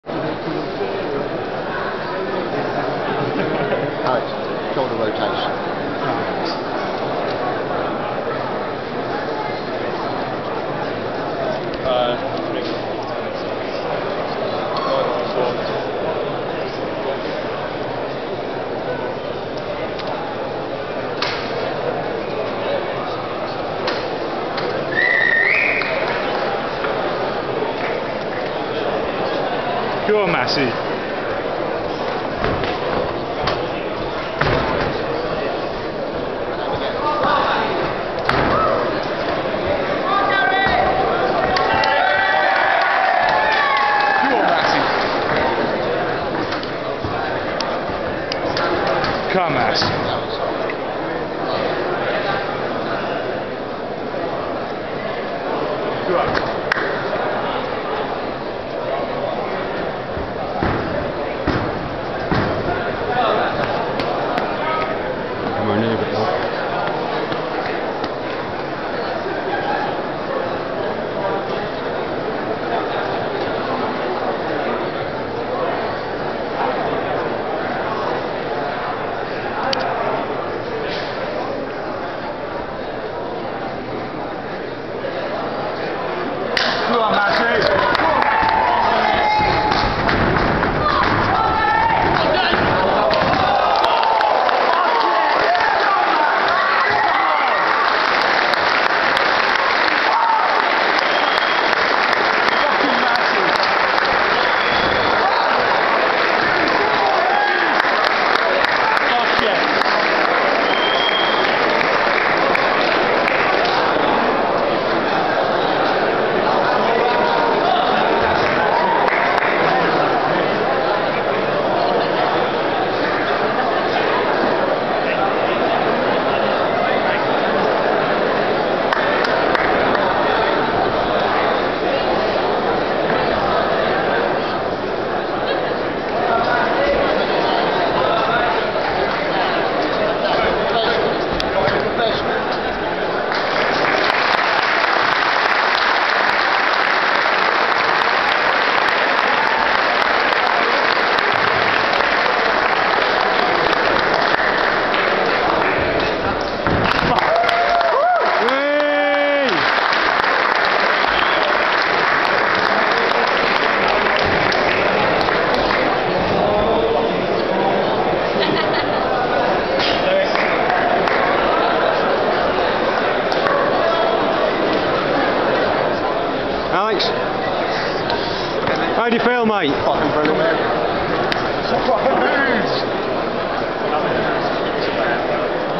Alex Massey Team Bushido 27-3-11.MOV
10 second KO at the TakeDown 3 event in Hyde, Cheshire